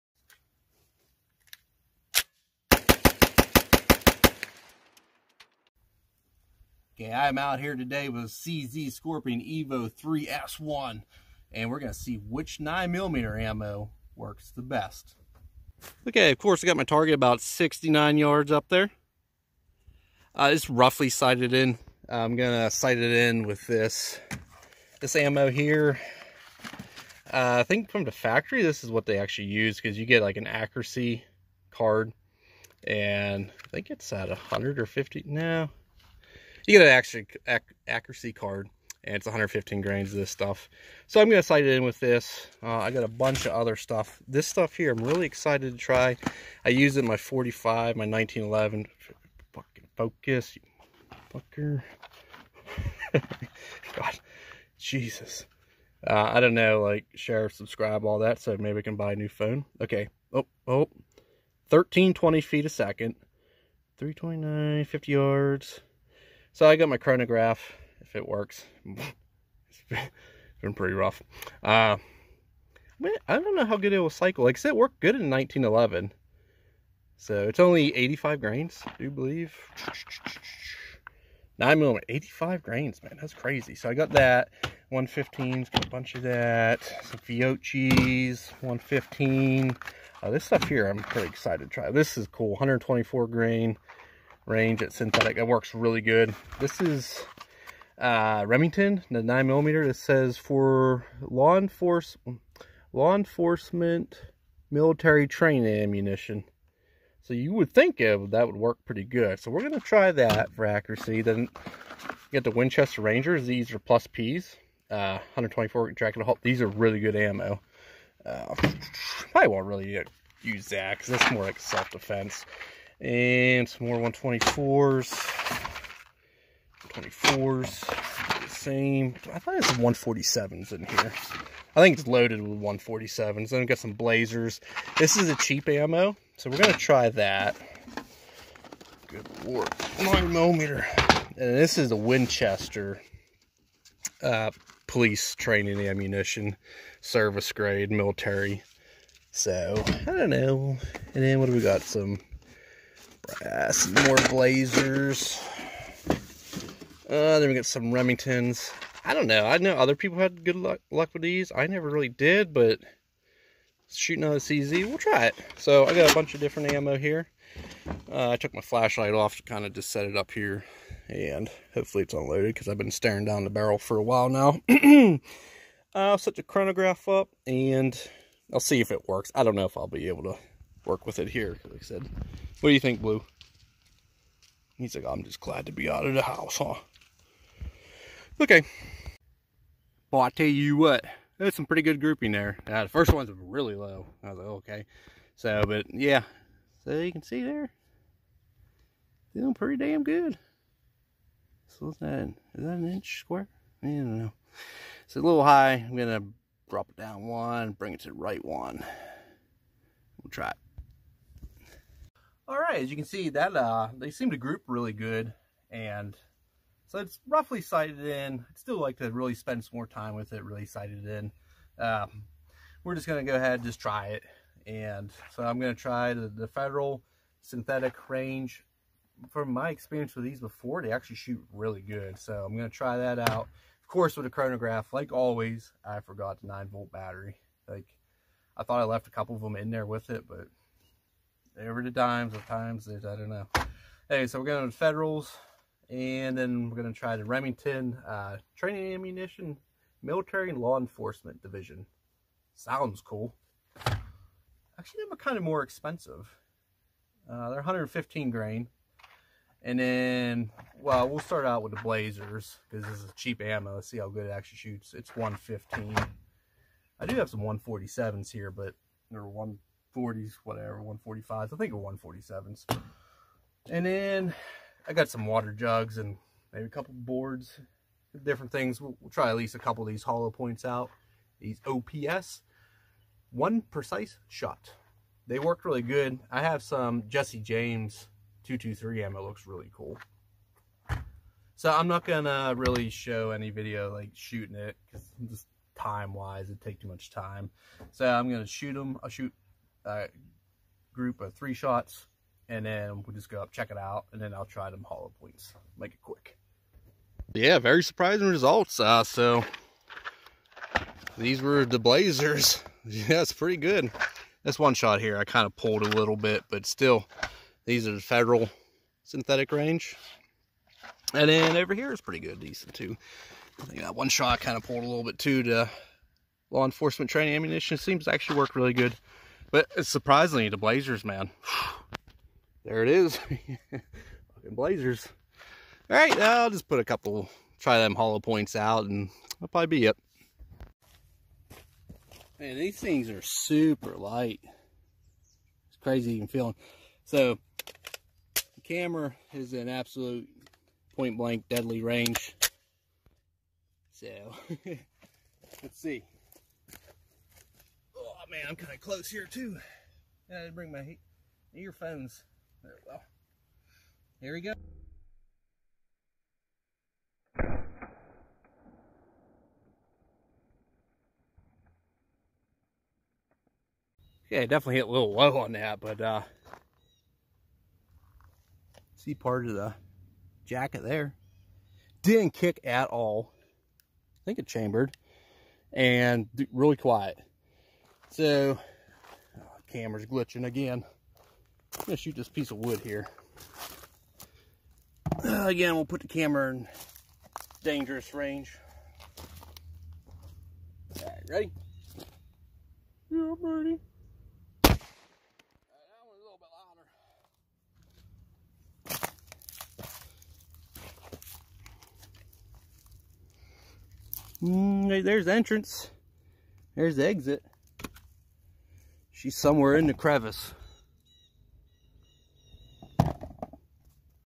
The Best 9MM Ammo!
trying out some different 9mm ammo with the cz scorpion